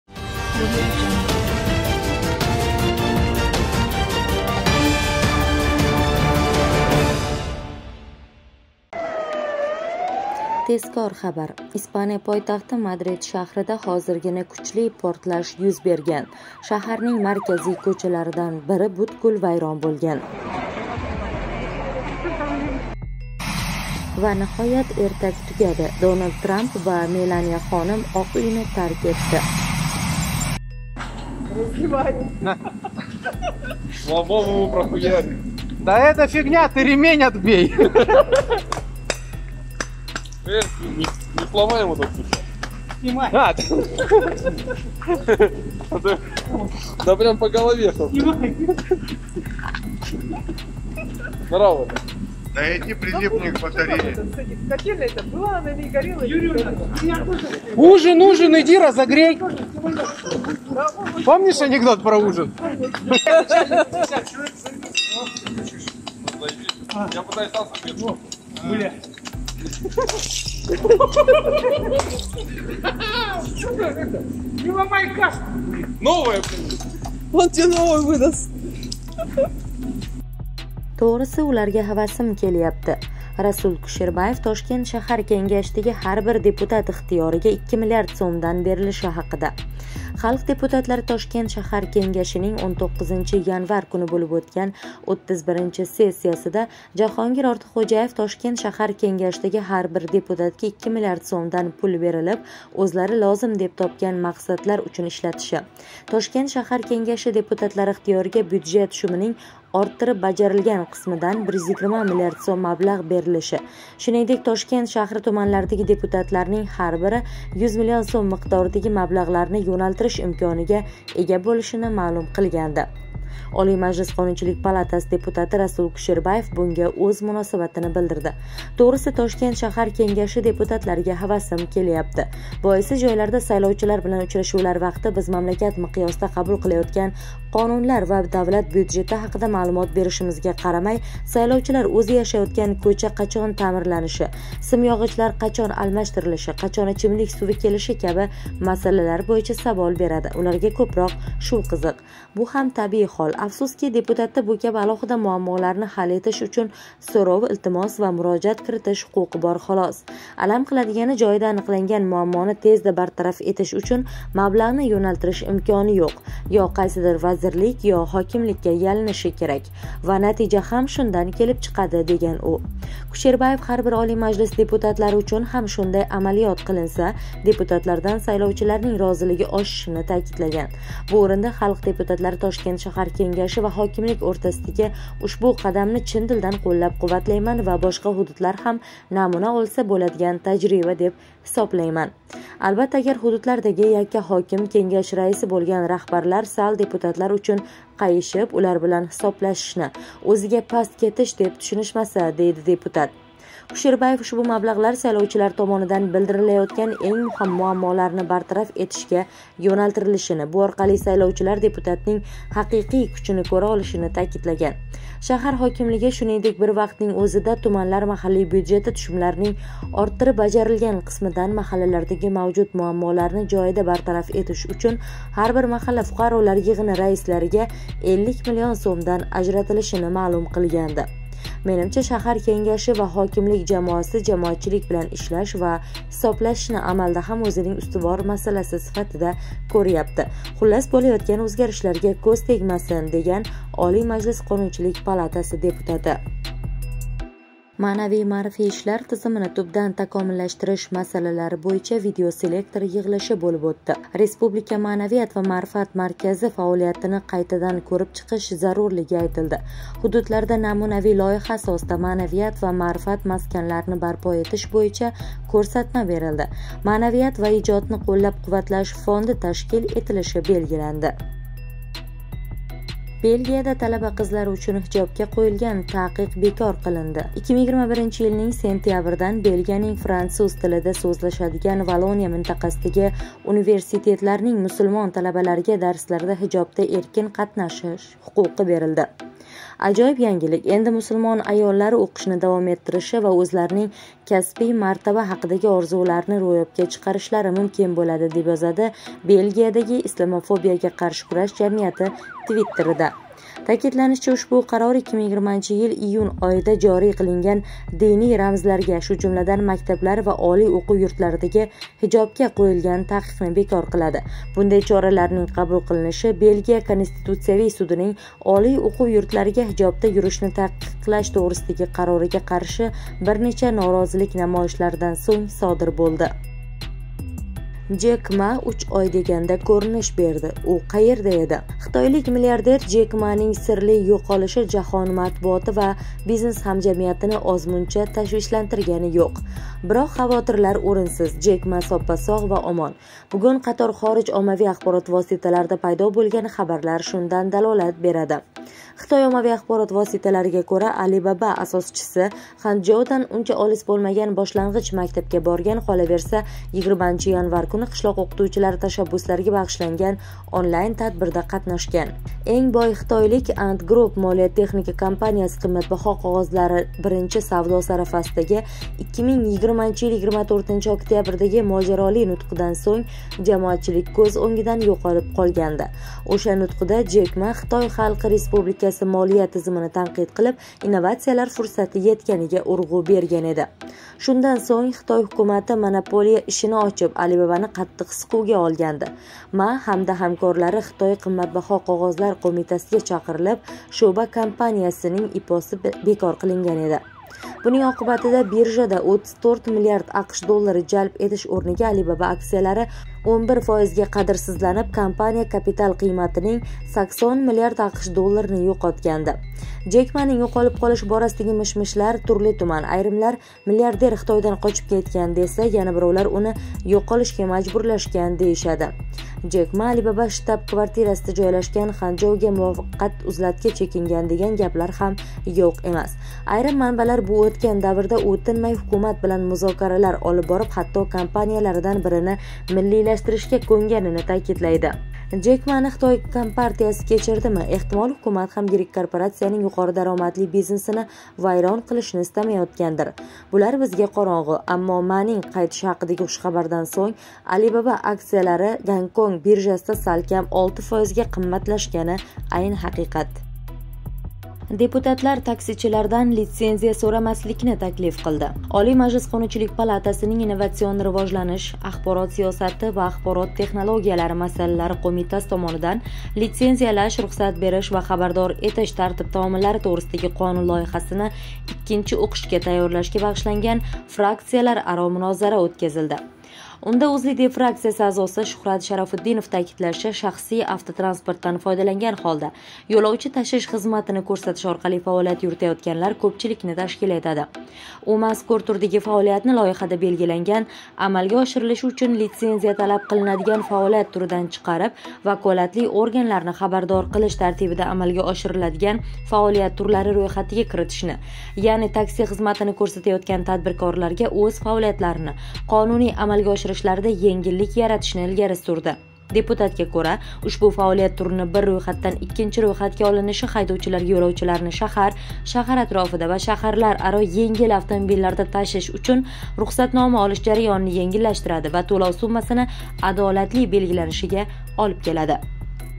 РАСУЛ КУШЕРБАЕВ КУТМАГАНДИ.ШАВКАТ МИРЗИЁЕВ ХАБАРИ .ИСПАНИЯ ПОЙТАХТИ ДАБДАЛА БУЛДИ...
Маънавий-маърифий ишлар тизимини тубдан такомиллаштириш масалалари бўйича видеоселектор йиғилиши бўлиб ўтди.
Маънавият ва маърифат маркази фаолиятини қайтадан кўриб чиқиш зарурлиги айтилди.
Ҳудудларда намунавий лойиҳа асосида маънавият ва маърифат масканларини барпо этиш бўйича кўрсатма берилди.
Маънавият ва ижодни қўллаб-қувватлаш фонди ташкил этилиши белгиланди.
Состоялось видеоселекторное совещание по вопросам коренного совершенствования системы духовно-просветительских работ.
Отмечена необходимость пересмотра деятельности Республиканского центра духовности и просветительства.
Дано указание построить в регионах дома духовности и просвещения на основе типового проекта.
Будет создан Фонд поддержки духовности и творчества.